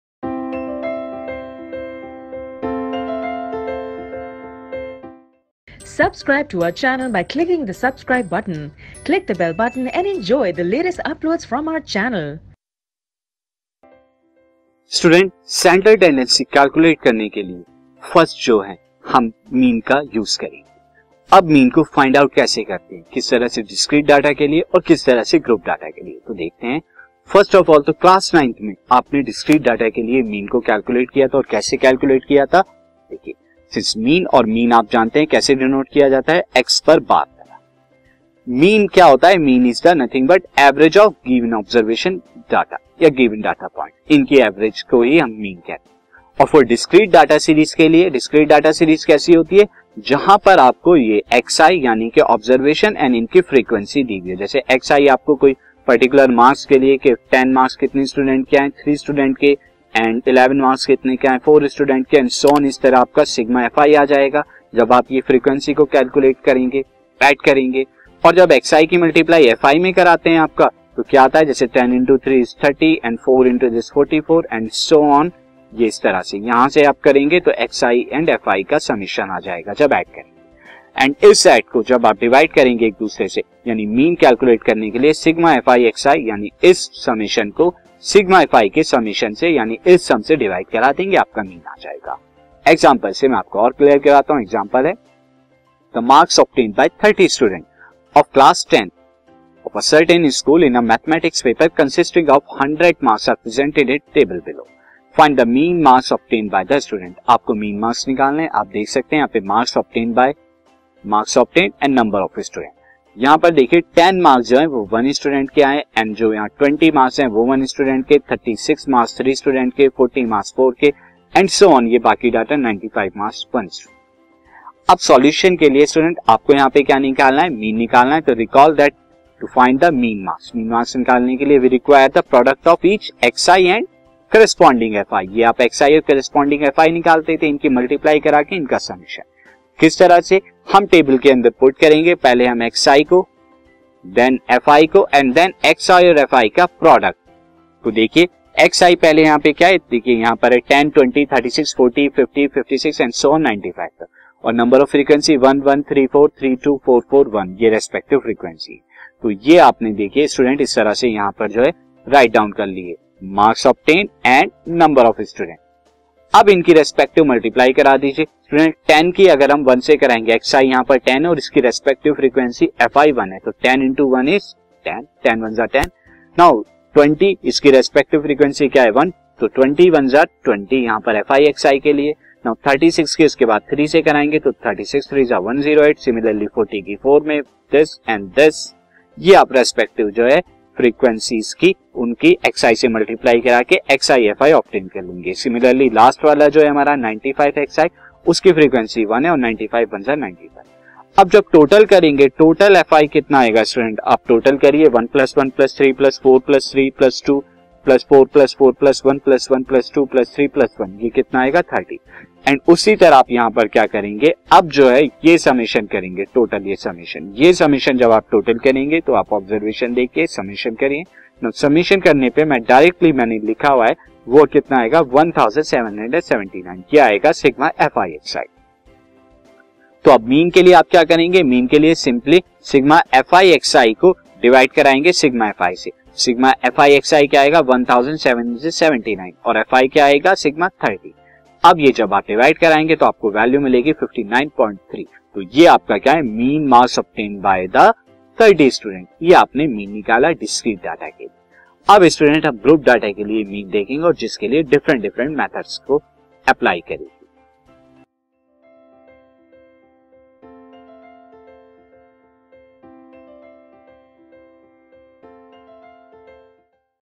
An Introduction to the concept of Central Tendency
This video is a part of the series for CBSE Class 10, Maths demo videos for the chapter “Statistics.” In this series, we have completed all the concepts, examples and the entire NCERT exercises. It is the part of an initiative taken to provide quality online education to students free of cost on YouTube.